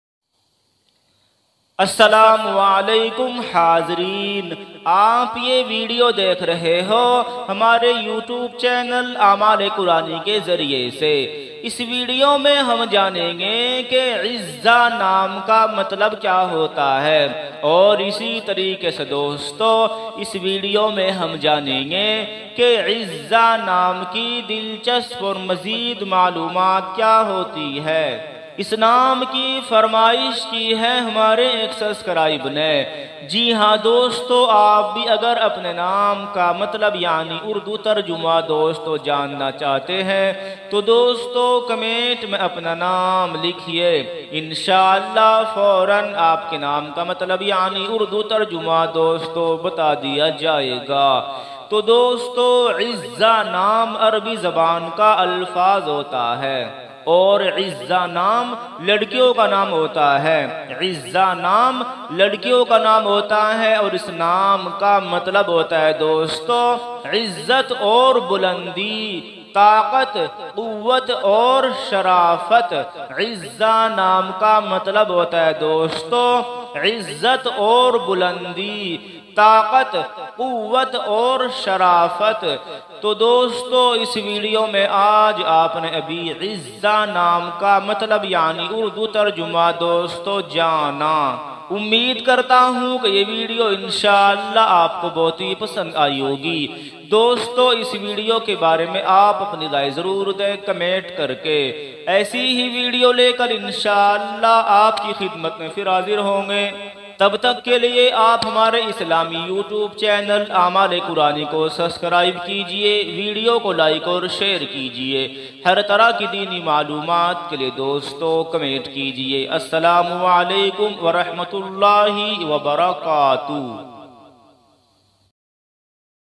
Izza Name Ki Meaning In Urdu | Izza Name Ka Matlab Kya Hota Hai
👉  Assalam Walekum Dosto Istaqbal Hai Apka Hamare Youtube Channel Aamale Qurani Me Aaj Ki Is Video Me Aap Janenge ( Izza )  ( इज्जा )  ( عزہ ) Name Ka Matlab Kya Hota Hai 👇